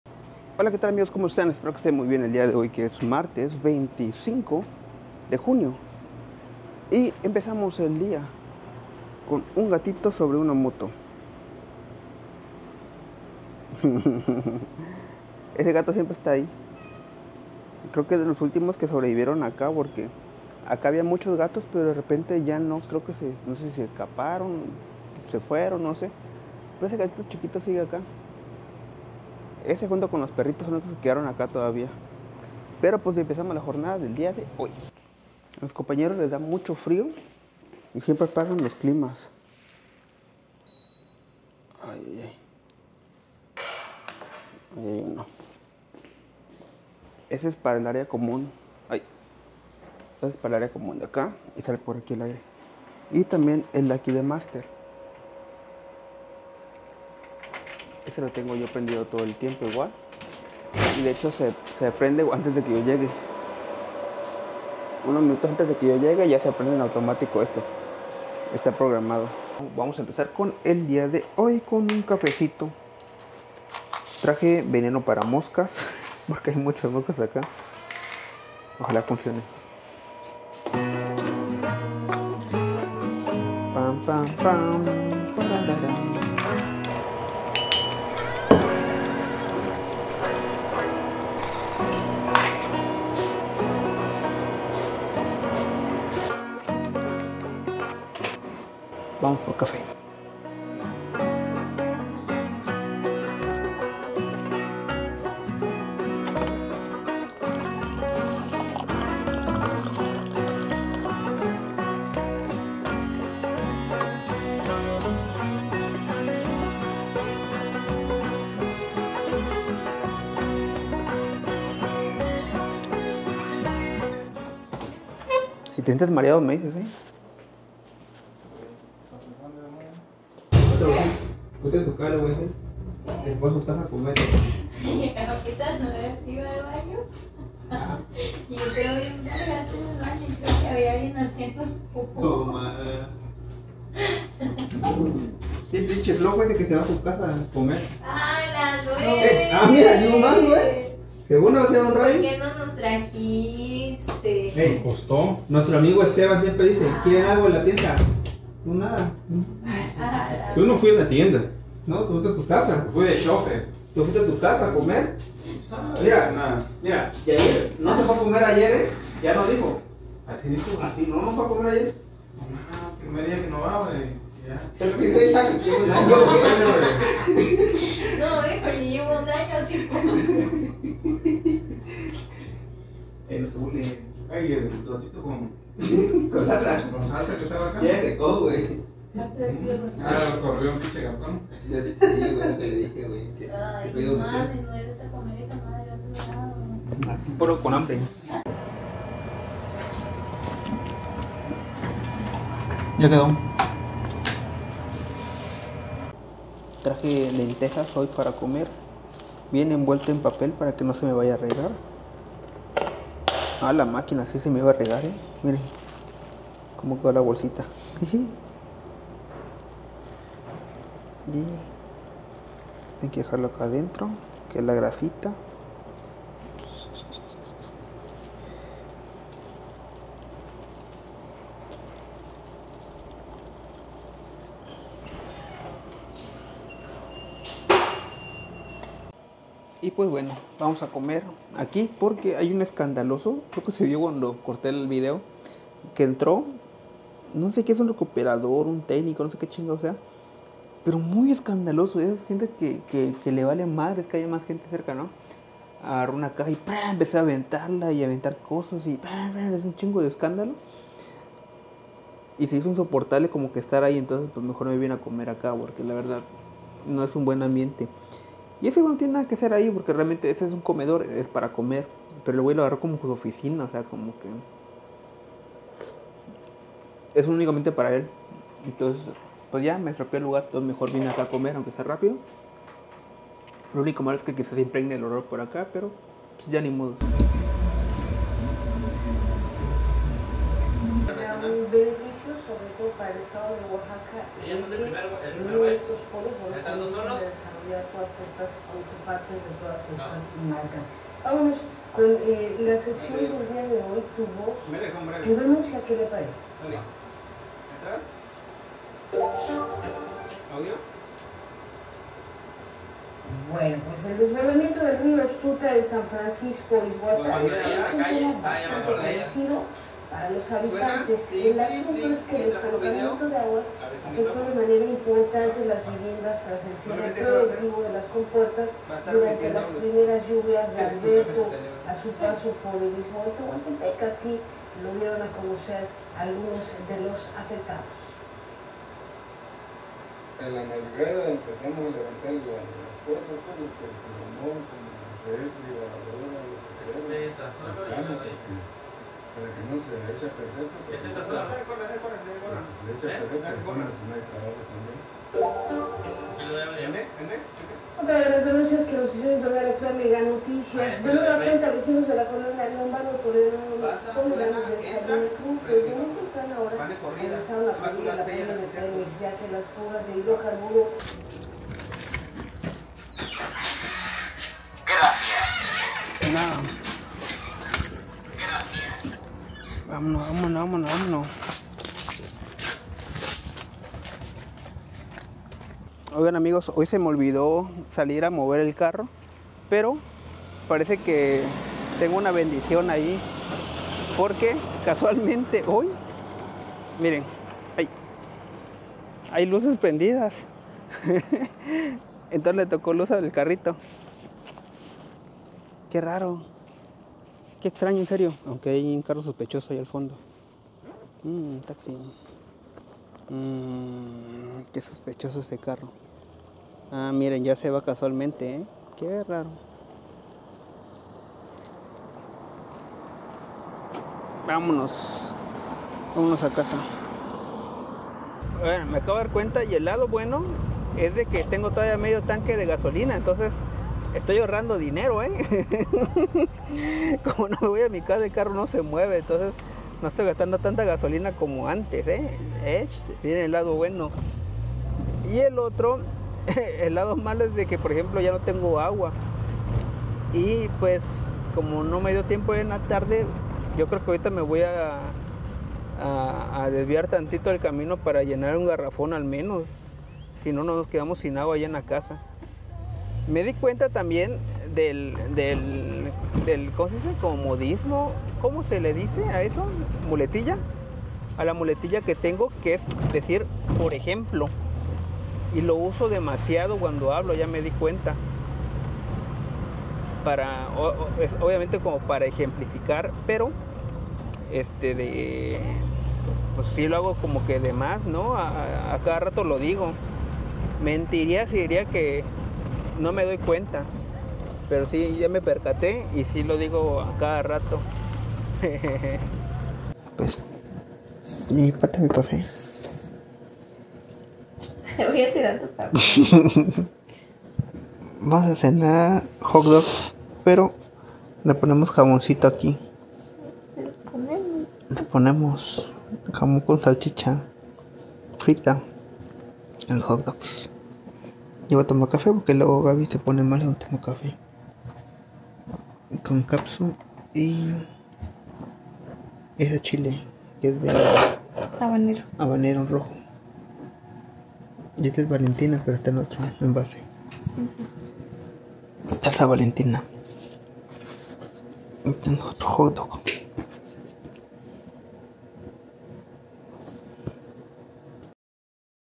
VIDEO DIARIO 25-06-2024
Ya pasó el segundo día de esta primera semana como editor, comí lentejas en el trabajo, menos tarde que ayer, vamos agarrando ritmo de a poco, el café no puede faltar, se me olvidó mover el carro por la noche y había un carro sospechoso cerca, al llegar a casa cene Hot Dog con café, Video Diario del 2024 de alguien que le gusta la Producción con una Osmo Pocket 3 en Salina Cruz, Oaxaca